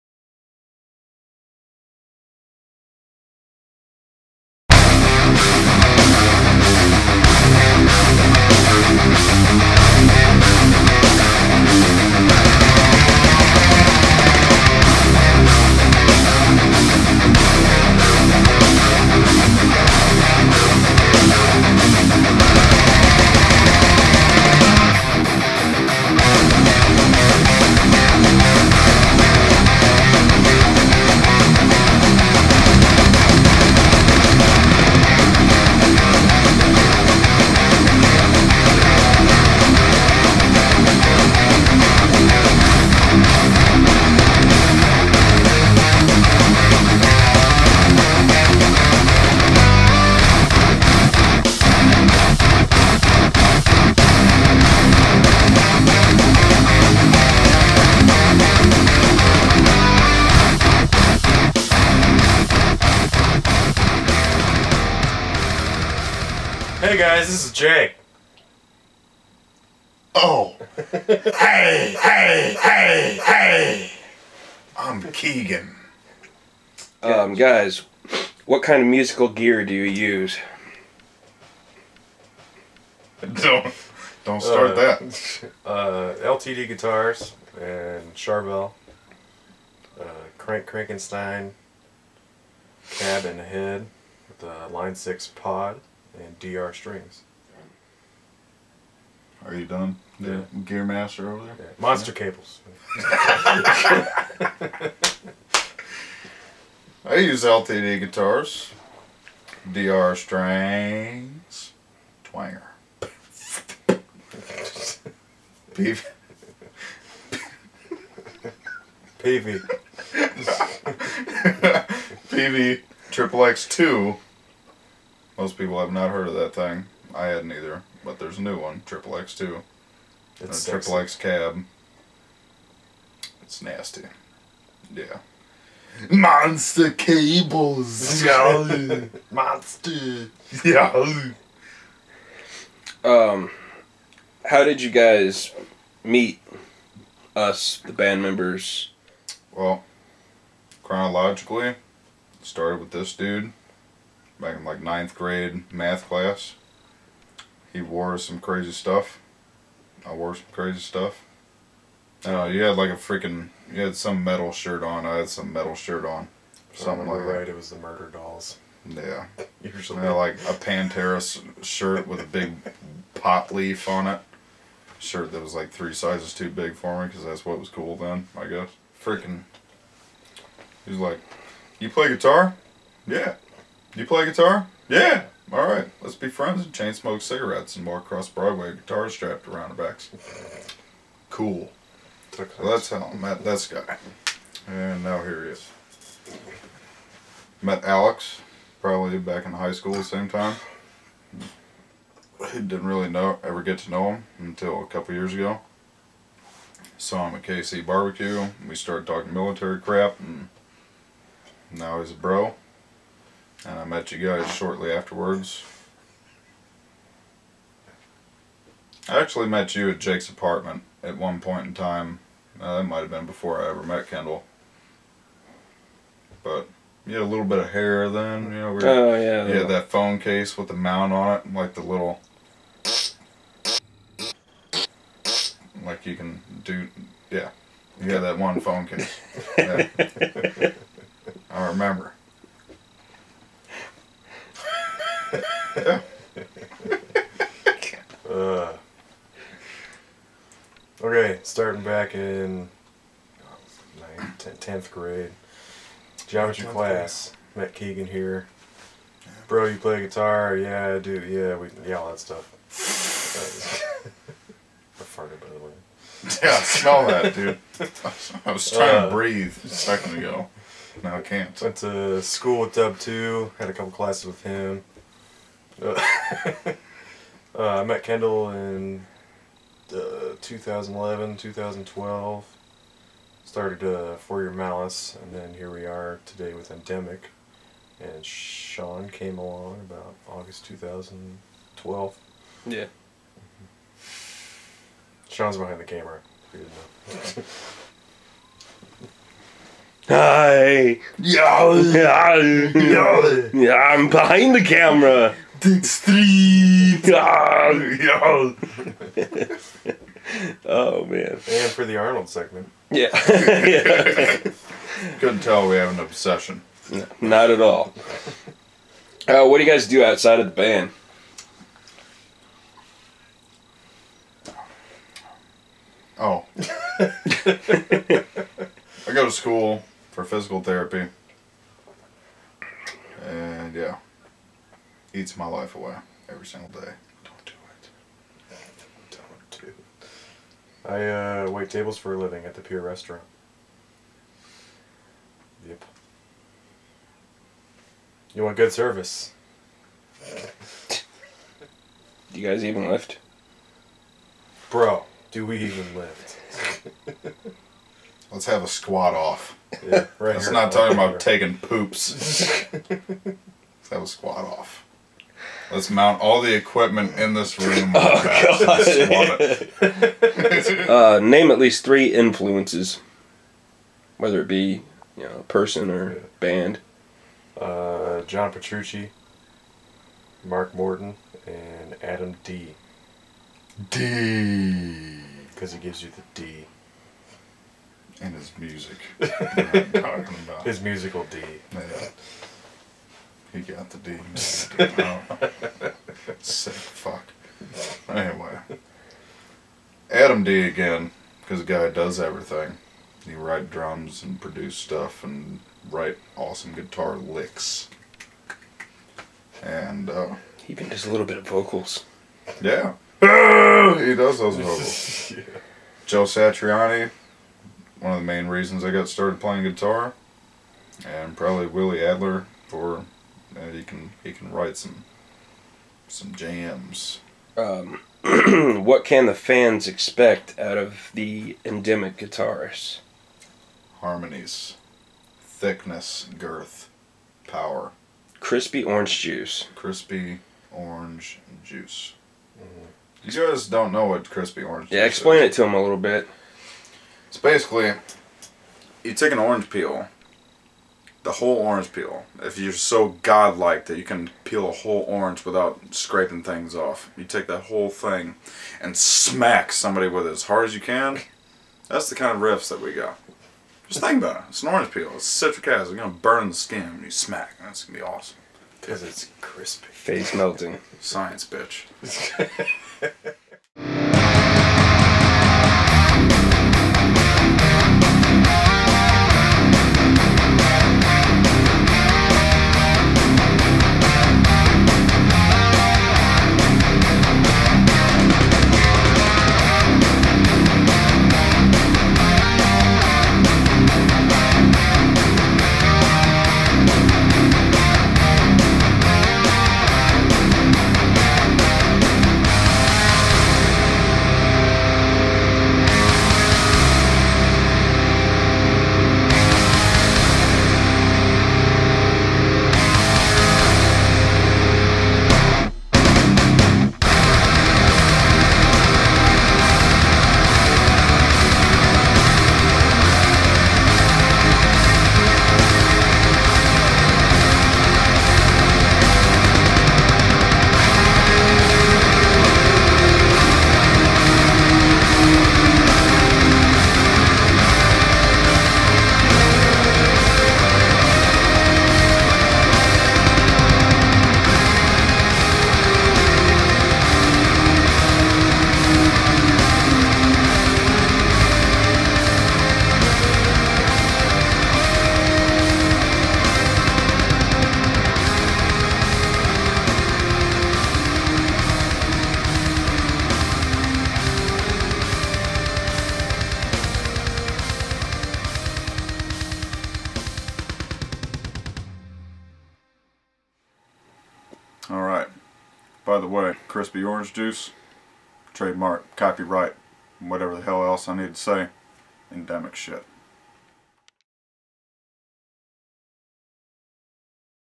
Endemic Vlog #2
Part 2 in our vlog series for the winter! This one features an impromptu interview with Keegan and Jake. There is also a play through snippet of one of our new tracks as well! Watch and enjoy!